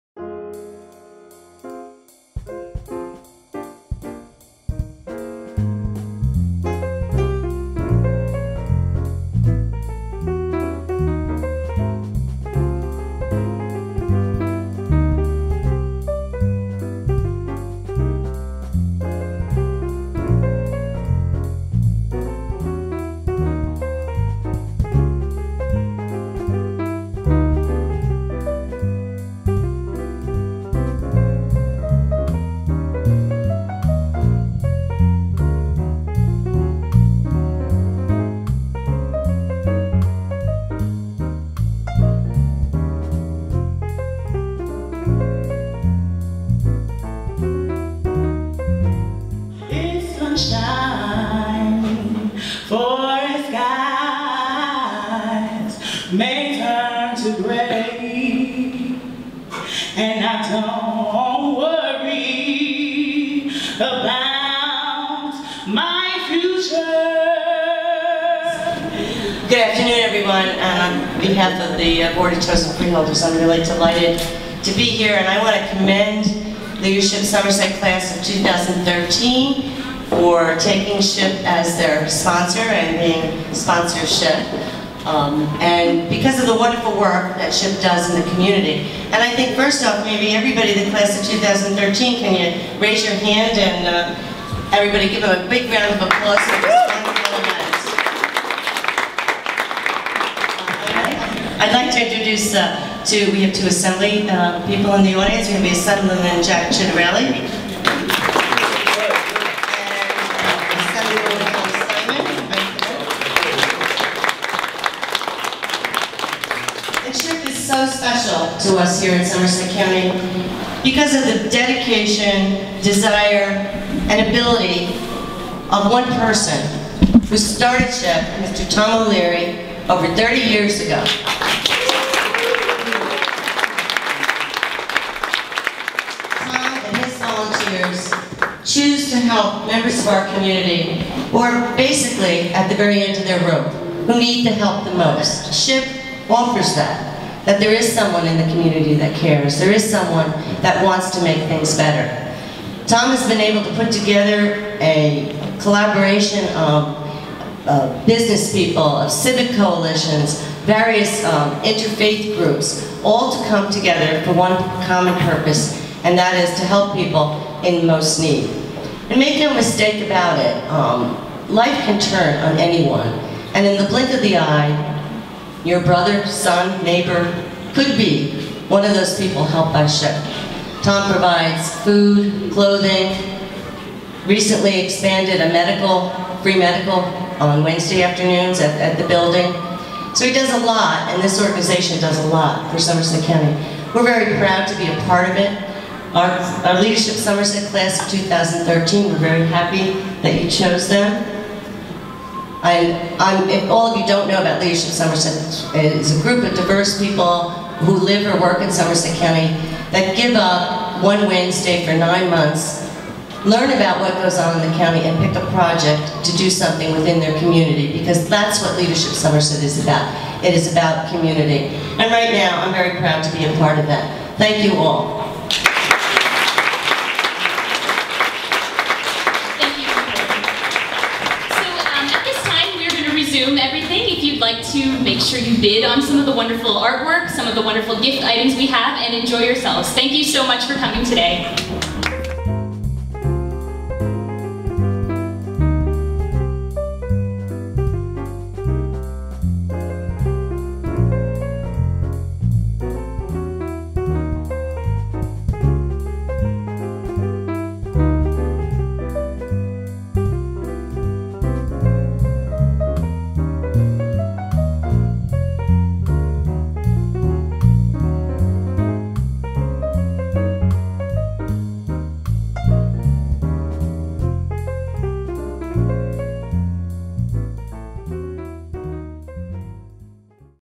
sponsorSHIP2013
Samaritan Homeless Interim Program
Oct 12, 2013
at St. John's Episcopal Church in Somerville NJ
Art Show and Entertainment with a speech by Freeholder Patricia Walsch.
The Sponsor SHIP event was a part of the Leadership Somerset Program 2013. The event featured the work of twenty-seven local and regional artists including internationally known cartoonist, Patrick McDonnell. Caricatures drawn by the art students of Immaculata High School, Gift auction baskets and refreshments for the evening were aided by local stores and restaurants. Musical accompaniment featured Jim Jaison and Daijiro Okada.